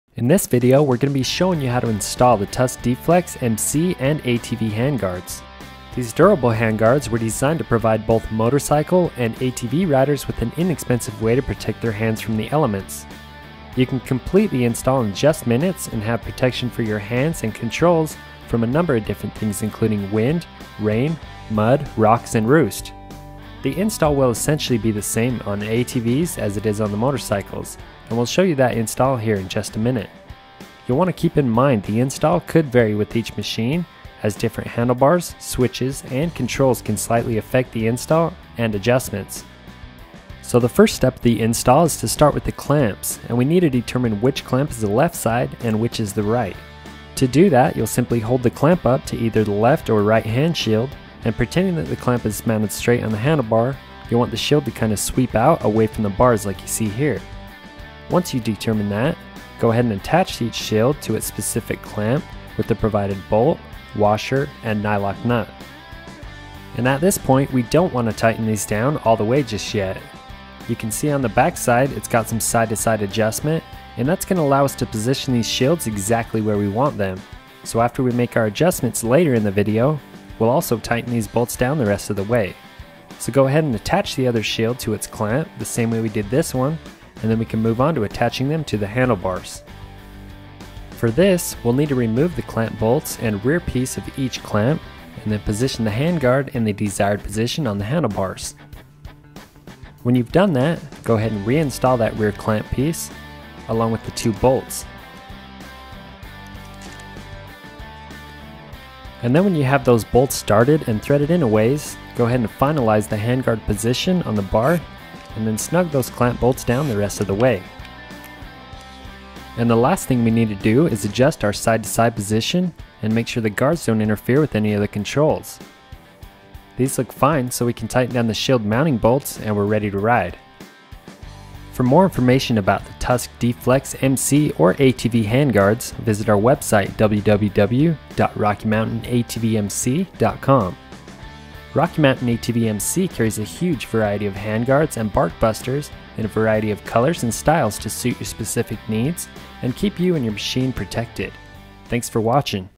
Tusk MX D-Flex MC & ATV Handguards Installation
The Tusk D-Flex Motorcycle and ATV handguards are an excellent upgrade for your machine! These handguards were designed to be an affordable set of hand shields that help protect your hands and controls from mud, water, rocks, and roost. The durable clamps are made of 6061 Aluminum and can easily be fitted to both 7/8" and 1 1/8" handlebars. The clamp design allows you position each hand shield into the desired position exactly, and independently from your levers and controls. The Tusk D-Flex Handguards are great for mc and atv applications not matter what type of riding you are doing. Whether you're racing motorcross or offroad, or you're just a weekend warrior and trailrider, these guards provide the hand protection that you need. These handguards can fitted onto just about any motorcycle or atv. Here you'll find a complete list of all the handguards and bark busters available for your machine!